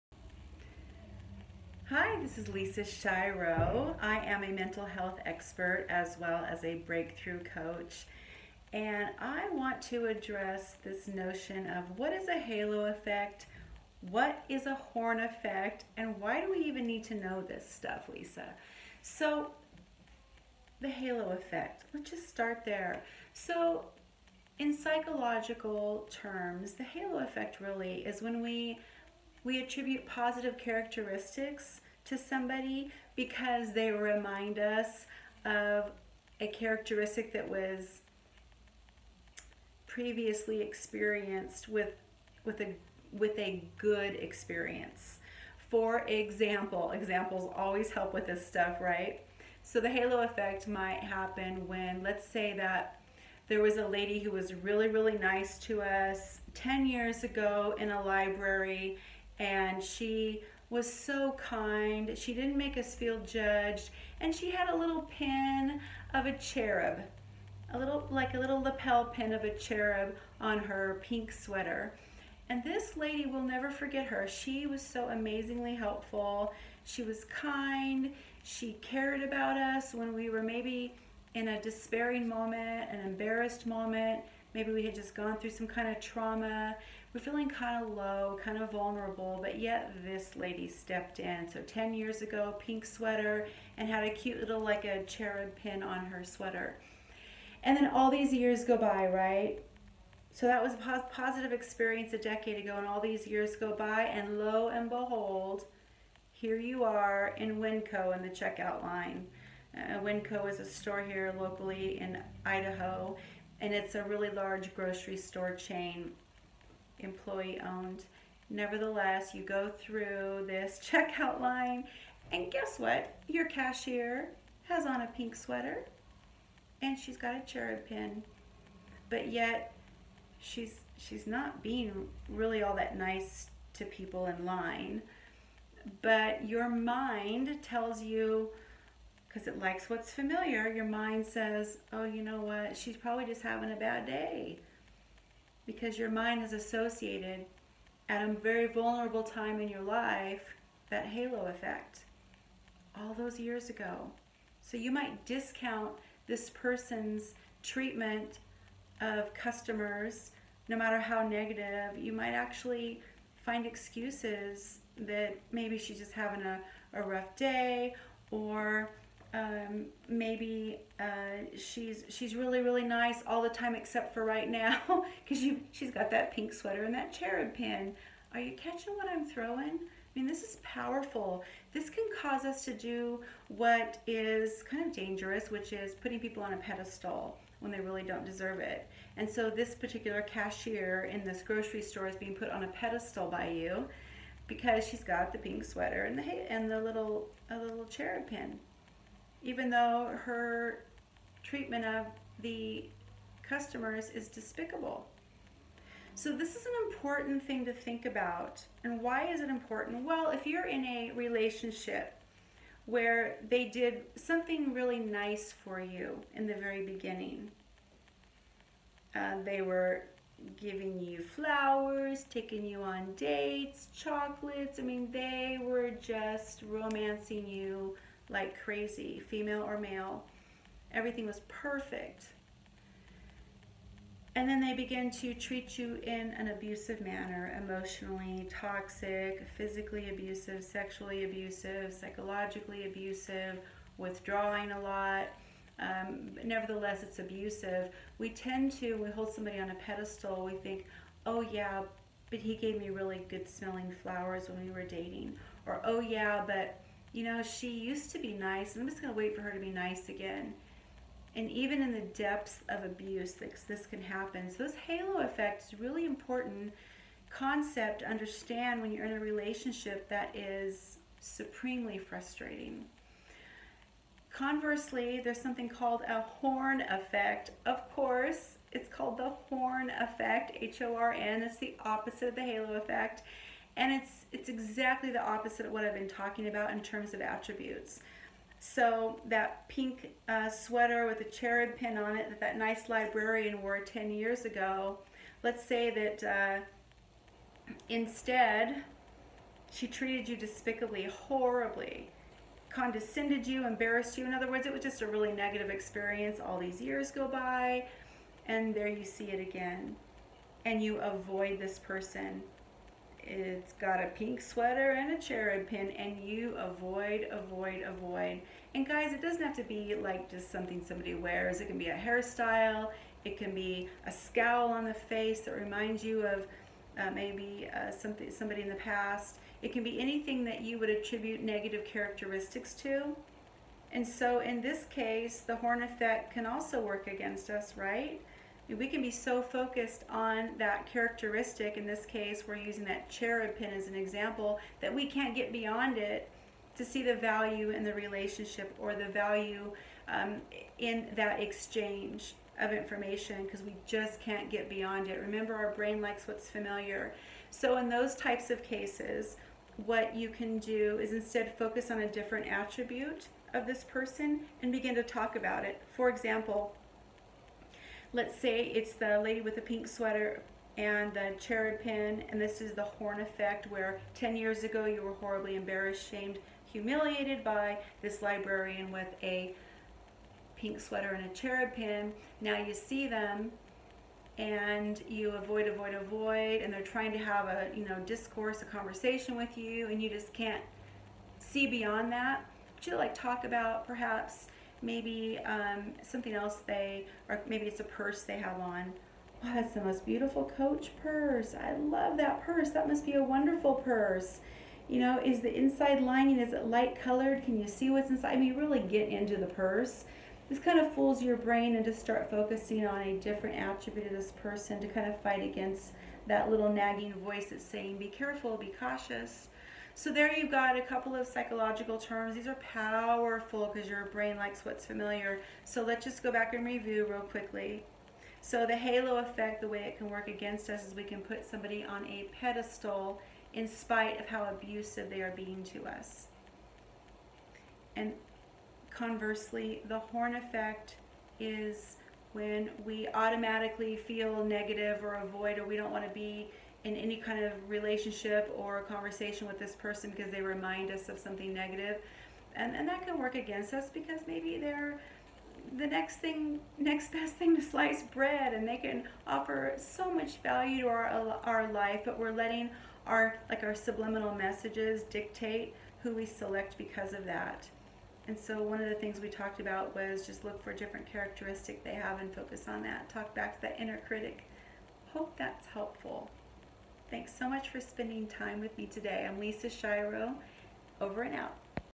Halo Effect vs. Horn Effect
Lisa Schiro, a Mental Health Expert and Breakthrough Coach, teaches you the difference between Halo Effect and Horn Effect.

More importantly, she provides practical ways to overcome this strong subliminal message so that you can create improved interpersonal relationships.